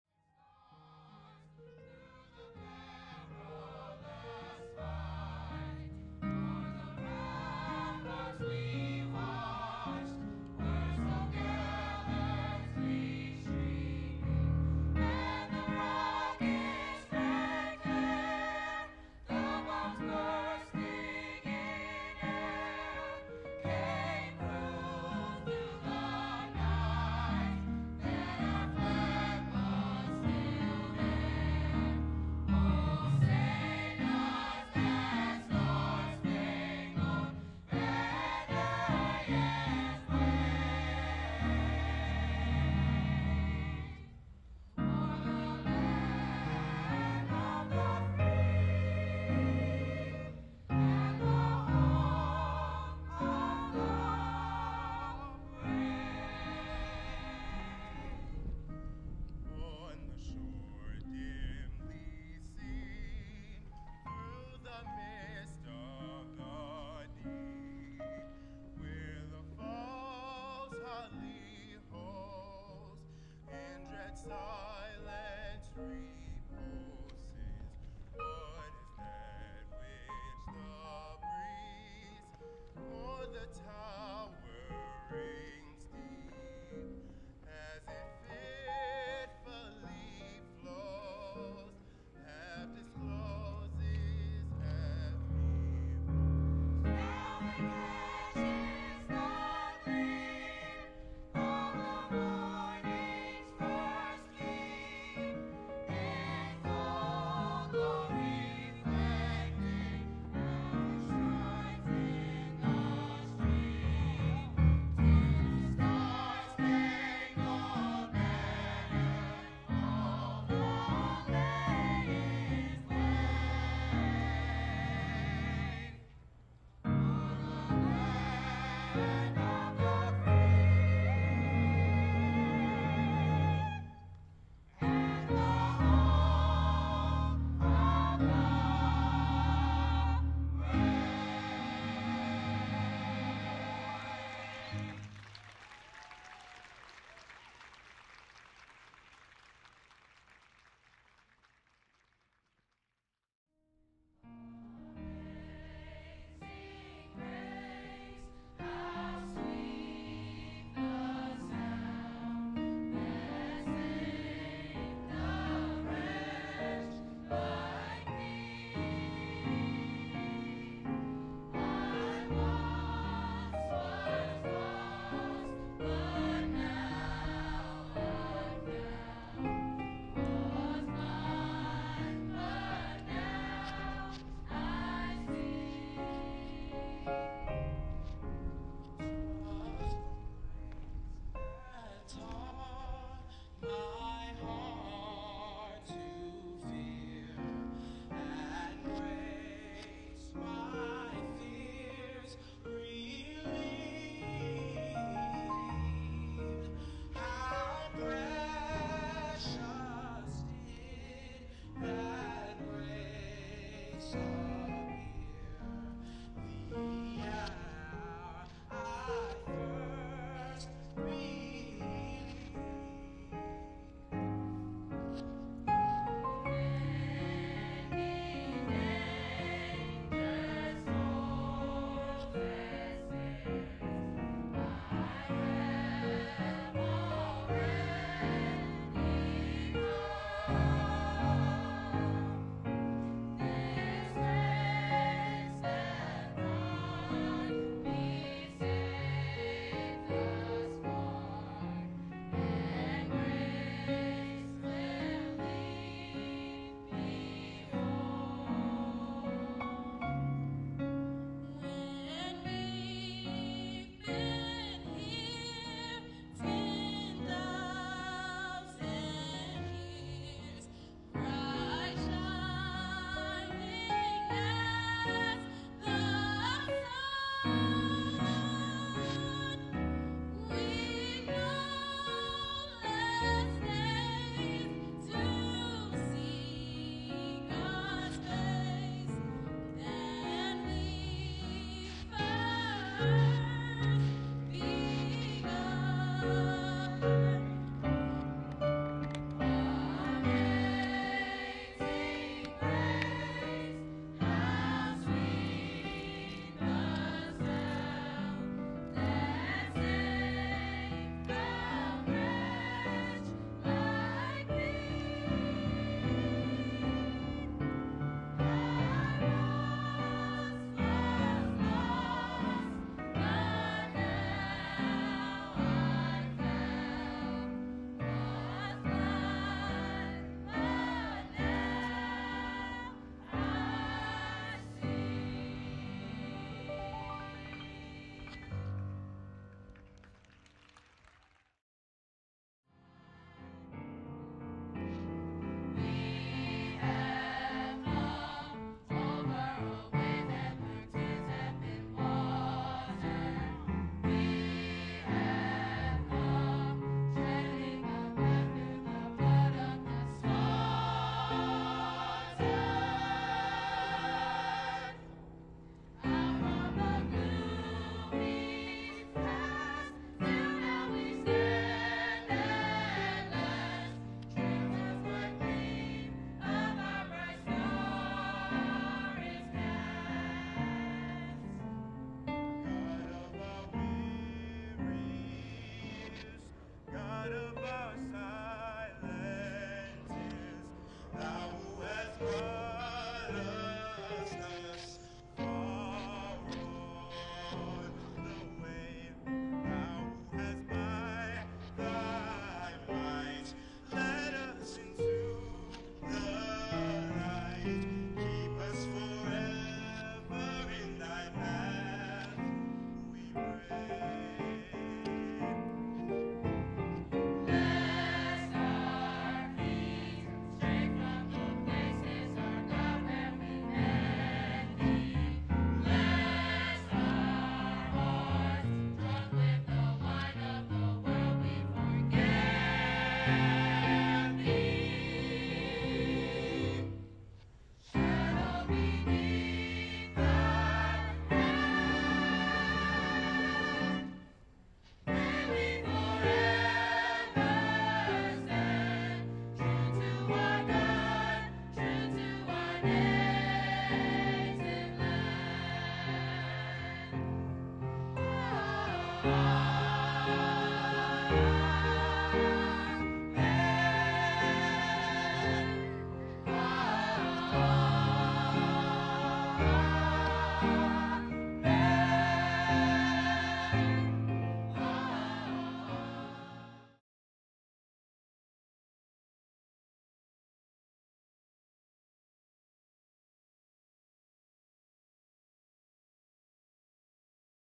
The Singing Sensation's Choir || Baltimore National Heritage Area
The Baltimore National Heritage Area has a mission  to promote, preserve and enhance Baltimore's historic and cultural legacy and natural resources for current and future generations.  The Defender’s Day 2023 program was intended to make the history of that date, September 12th, 1814 all about making the history of Fort McHenry relevant to our youngest citizens today.  BNHA is proud  to showcase the talent of our young people. We hope that all will be inspired to return to this place which is hallowed and preserved for all.